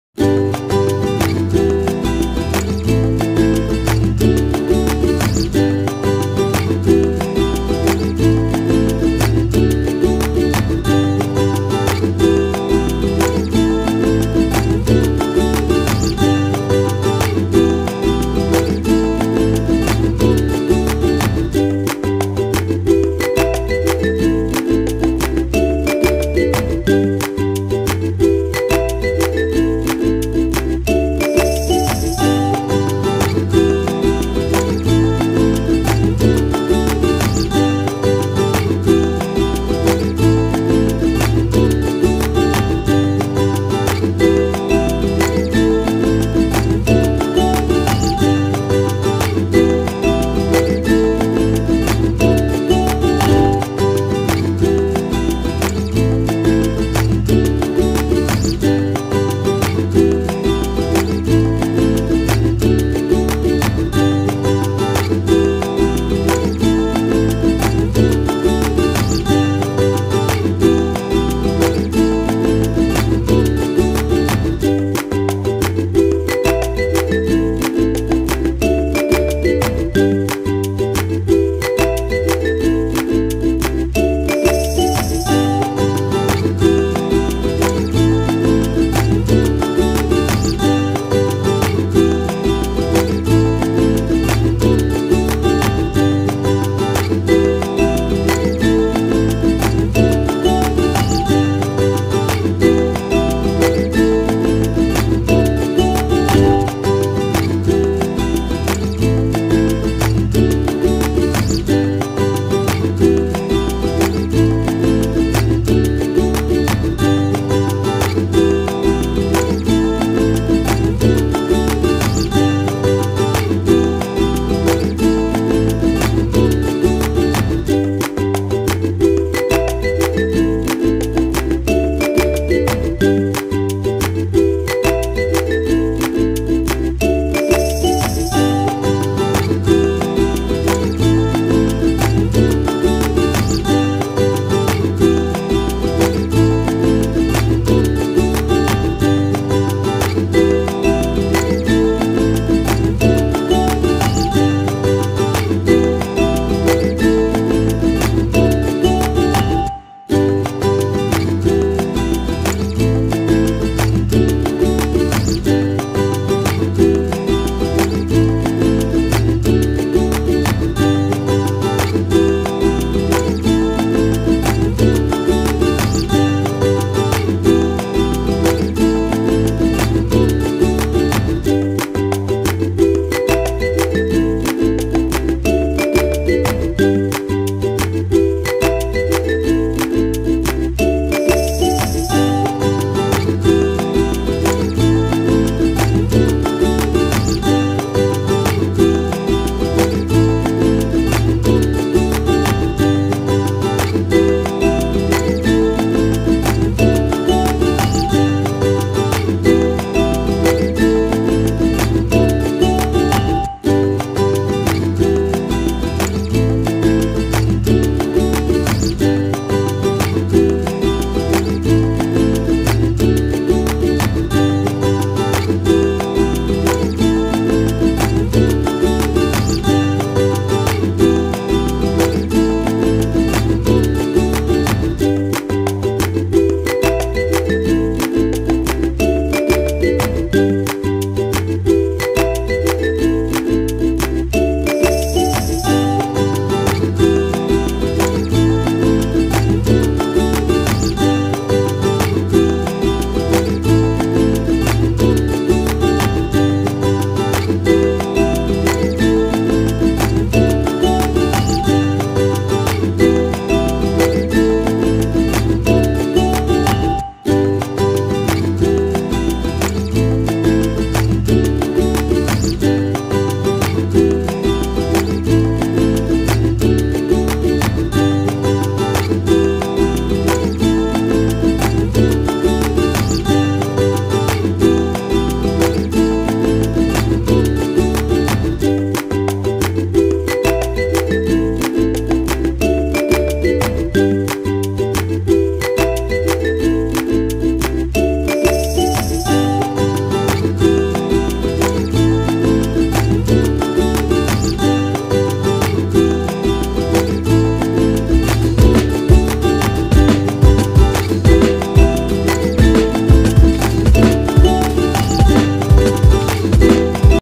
GINATAANG ALIMASAG AT HIPON SA GULAY | mabilis lang ito lutuin
So easy to cook the shrimp and crab with vegetables please try it